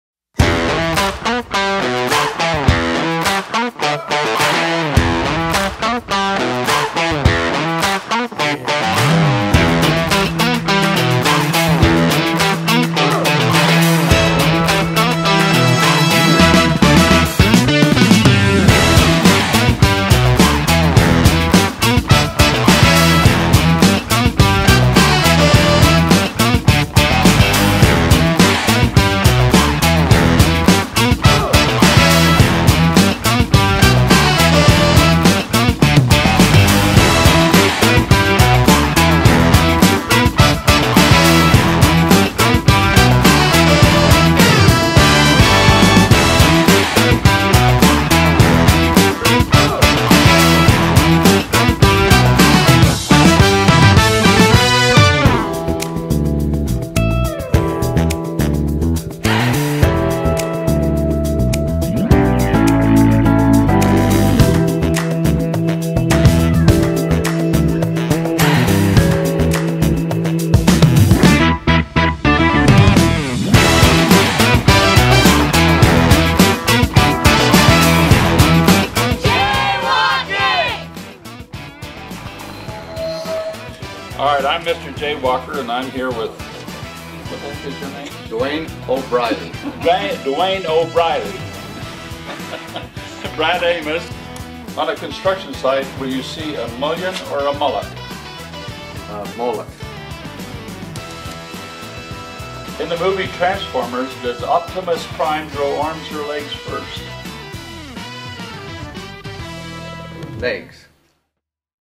Brad Amos - 40 Years at Wadman
Brad Amos. The Man, The Myth, The Legend! Brad is celebrating his 40th Year with Wadman Corporation! This man has endless stories that make you laugh so hard your face hurts! His very first project was in 1978 ... an elementary school and gym in Tremonton and he just went up from there! He has spent his whole adult life traveling all over the Western United States building for us... He spent years working in California and lived through riots, floods and fires. Brad continues to be a teacher, mentor and a great friend to all of us, not to mention he is our resident CPR expert! Congratulations on 40 years Brad! You are the BEST!